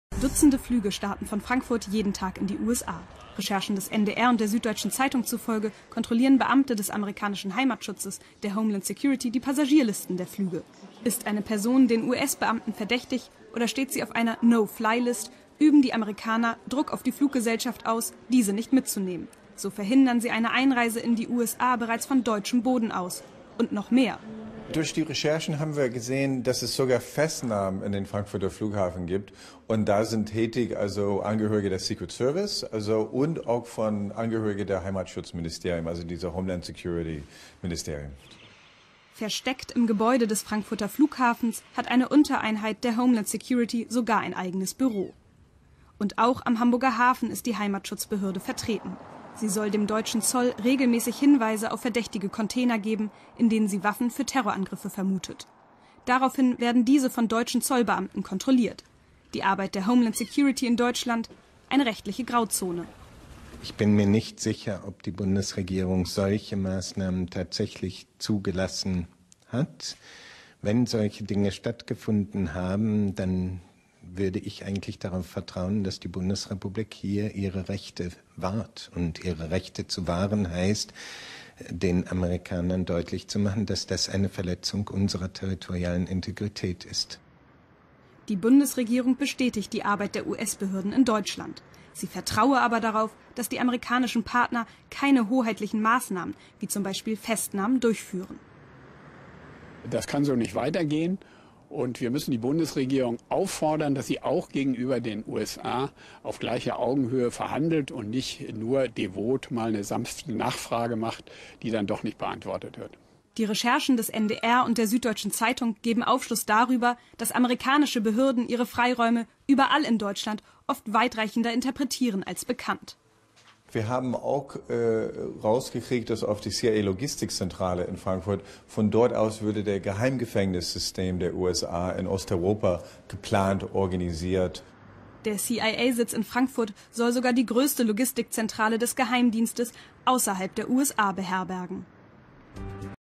Geheimer Krieg: Wie der Secret Service in Deutschland agiert [19.11.2013]
Nach Medienberichten über ausgedehnte Geheimdienst- und Militäroperationen der USA in Deutschland fordern die Grünen Aufklärung von der Bundesregierung. Diese soll erklären, ob es zuträfe, dass die USA hier einen "geheimen Krieg" führten.